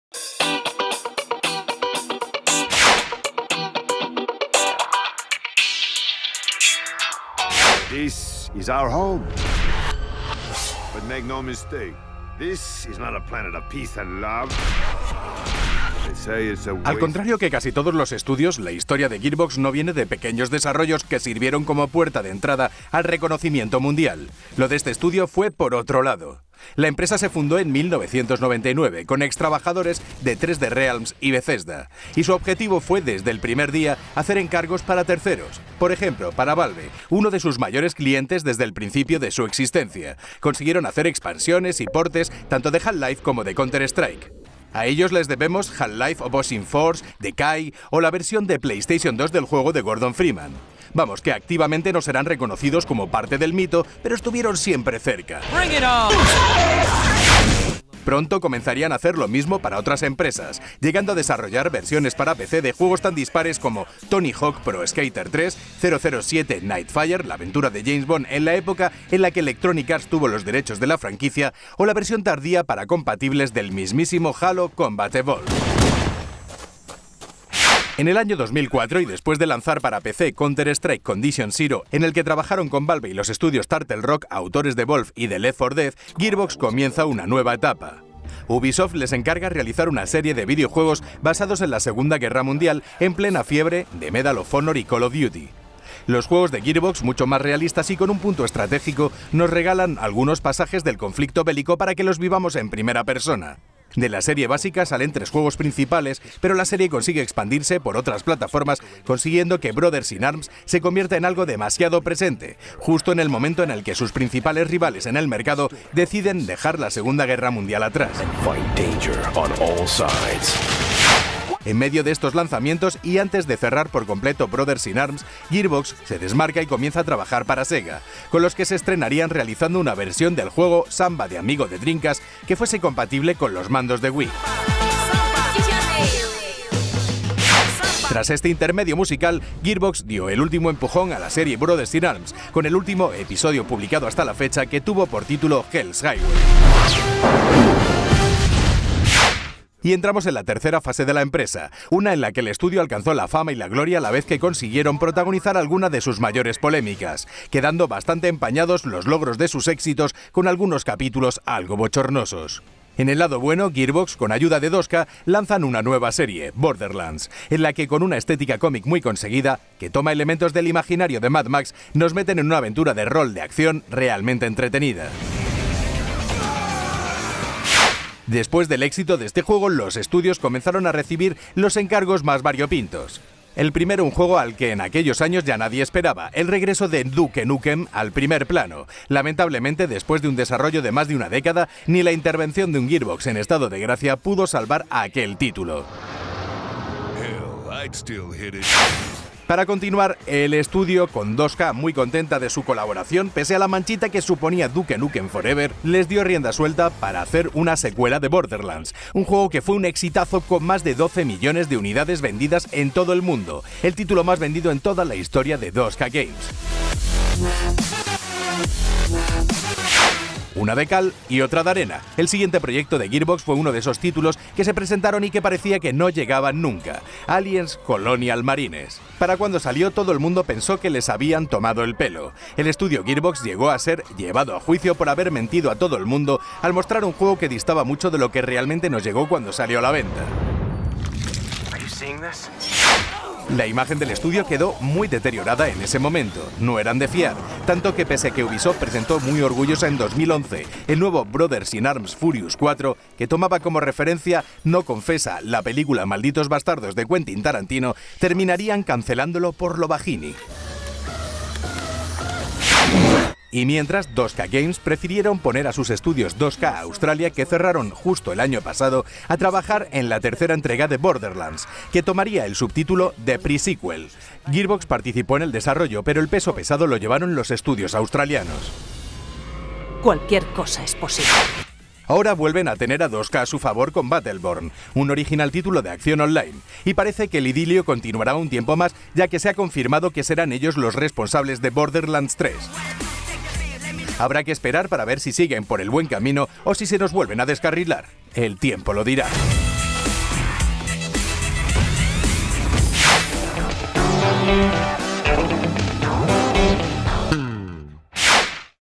La historia de Gearbox
Los creadores de Borderlands acaban de lanzar su nuevo juego, Battleborn, lo que aprovechamos para repasar su trayectoria, llena de luces y algunas sombras.